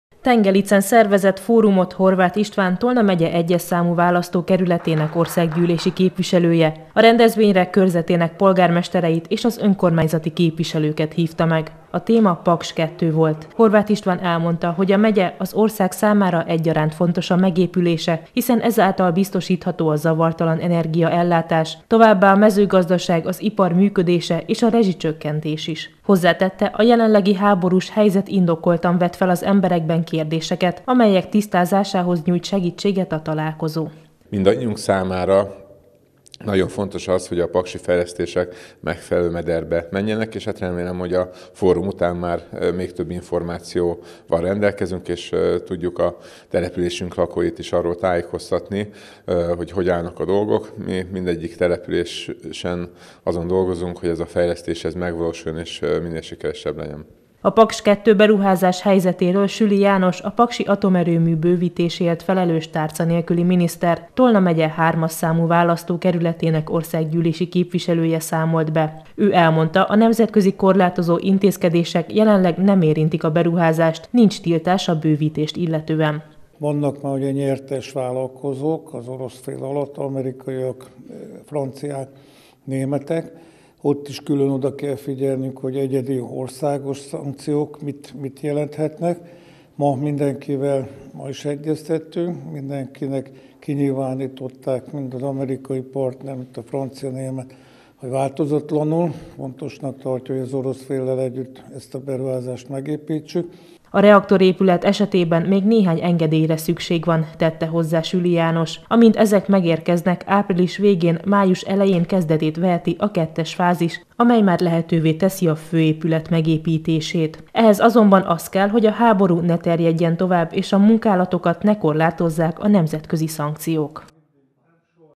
Paks 2 beruházás - Süli János tartott előadást a térség polgármestereinek Tengelicen
Fórumot szervezett Tengelicen Horváth István, Tolna megye 1-es számú választókerületének országgyűlési képviselője. A rendezvényre meghívást kapott Süli János, a Paksi Atomerőmű bővítéséért felelős tárca nélküli miniszter, Tolna megye 3-mas számú választókerületének országgyűlési képviselője, aki a Paks 2 beruházásról tartott előadást a térség polgármestereinek. Elmondta, a nemzetközi korlátozó intézkedések jelenleg nem érintik a munkálatokat, nincs tiltás a bővítést illetően.